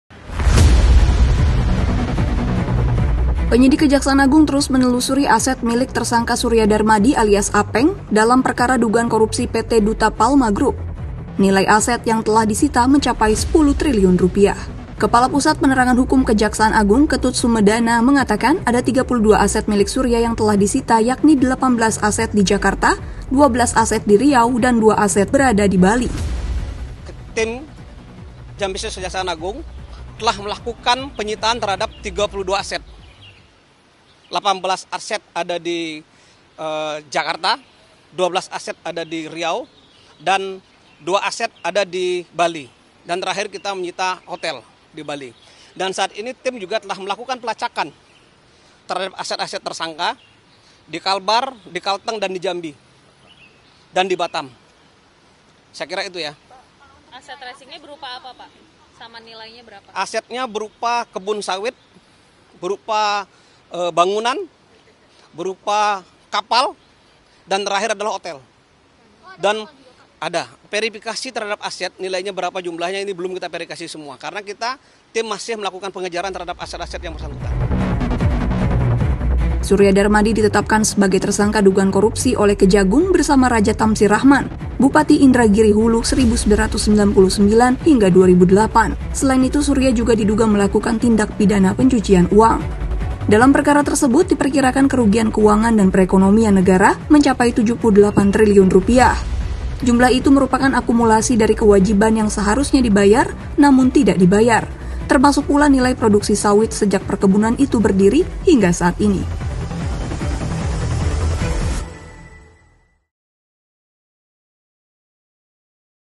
Kejagung Sita 32 Aset Surya Darmadi
Penyidik Kejaksaan Agung terus menelusuri aset milik tersangka Surya Darmadi alias Apeng dalam perkara dugaan korupsi PT Duta Palma Group. Korupsi ini diperkirakan merugikan keuangan dan perekonomian negara hingga Rp 78 triliun. Nilai aset yang telah disita mencapai Rp 10 triliun.